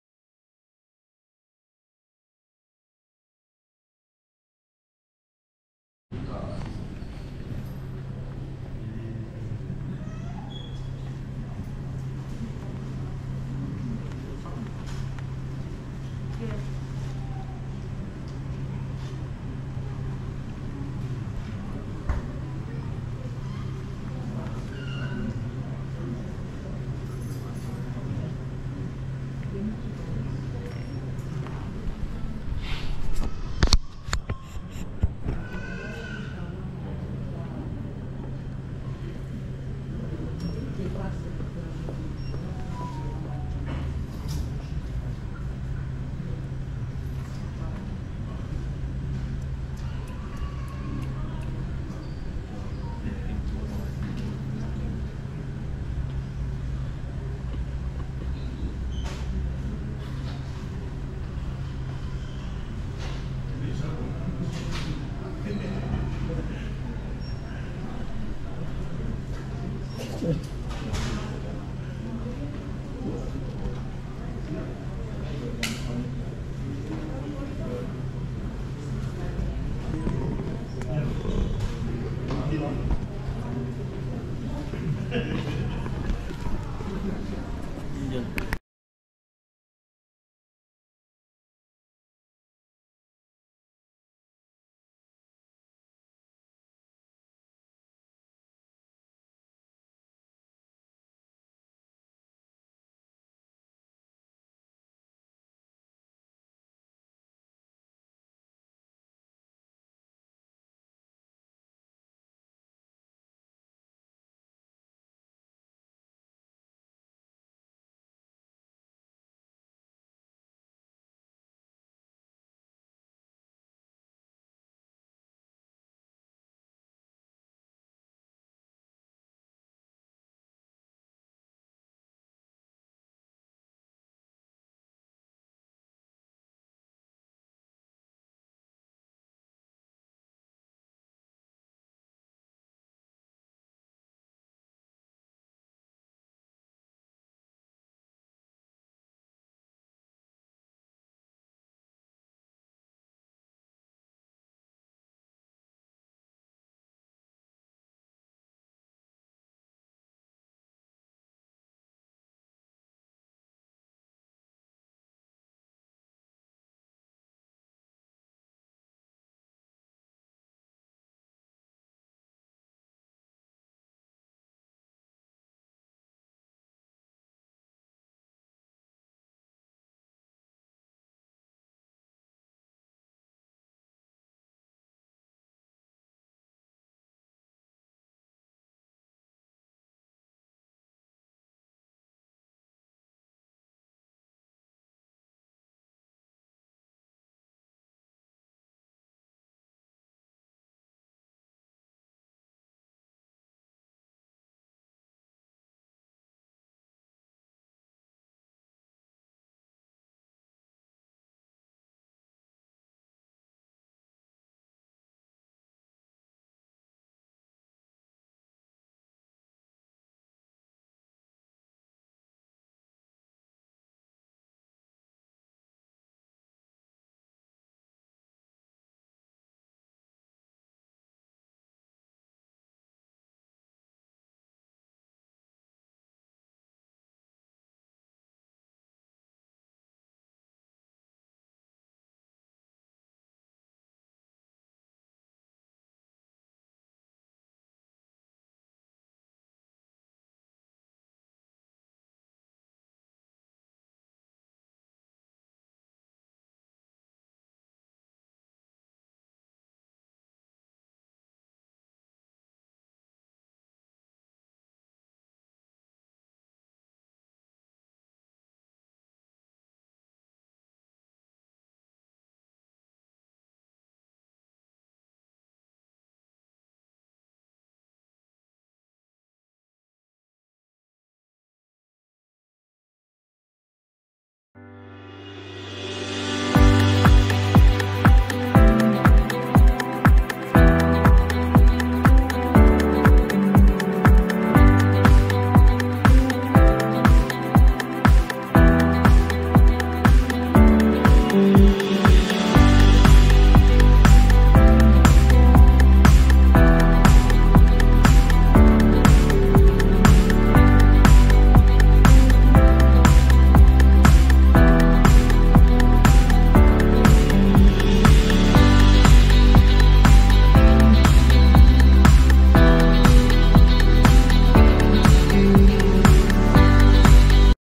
intercity bus experience
Experience Intercity buss in Munukao
the terminal was so clean and you can buy snacks and water inside while waiting for your
 bus.